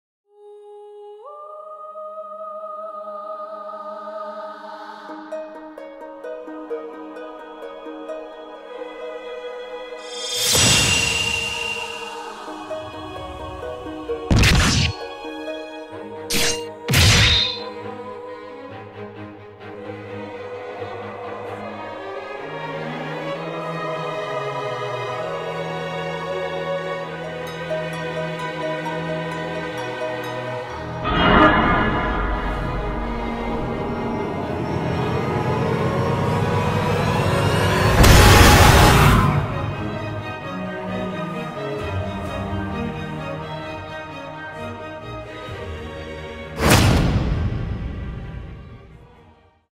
The Light Chronicles | Trailer | GachaPenguin
This series explains Penguin’s origins, how he met his friends, and how he got his powers.

Coming Soon... Somewhere in 2022

Composed by Noriyasu Agematsu (Spotify has it on Japanese, so idk if it’s correct)